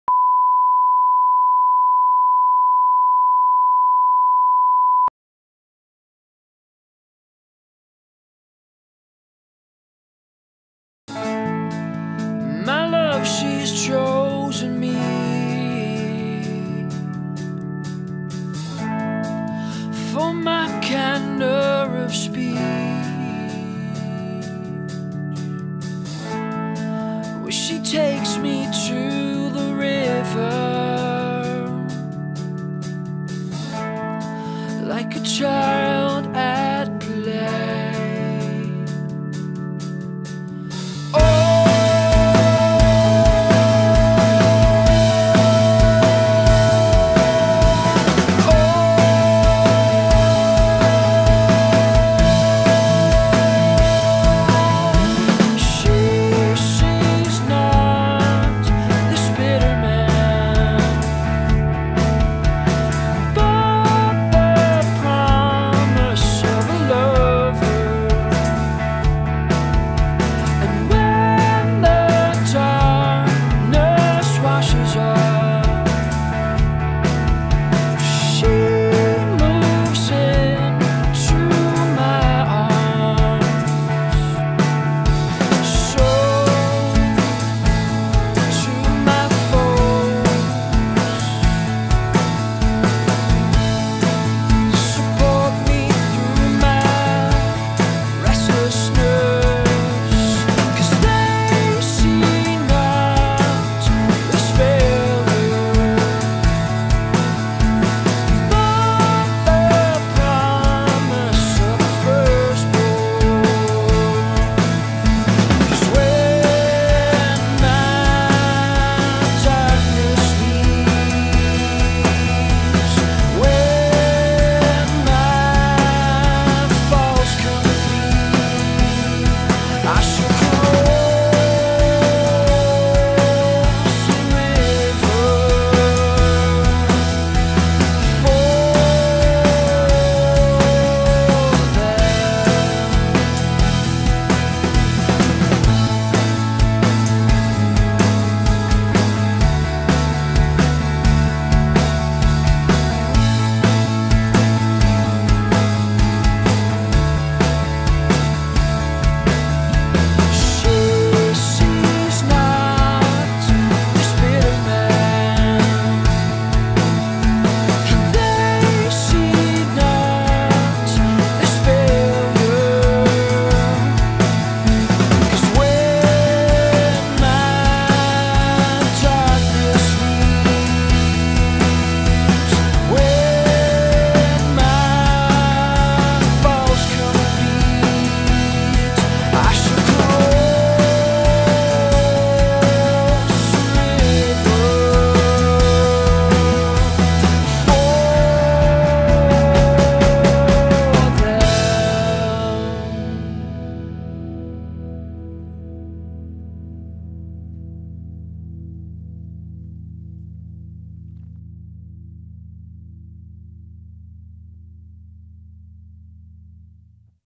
The Black Atlantic - Live @ Spitsbergen
Voor m'n werk heb ik vorig jaar opnames gemaakt voor de band 'The Black Atlantic' in studio Spitsbergen. Een (inmiddels oud) collega heeft de montage gedaan en dit is het resultaat.

PS. het intro met de gekleurde balken zou eigenlijk nog weg moeten, maar 'k was wat lui ;-)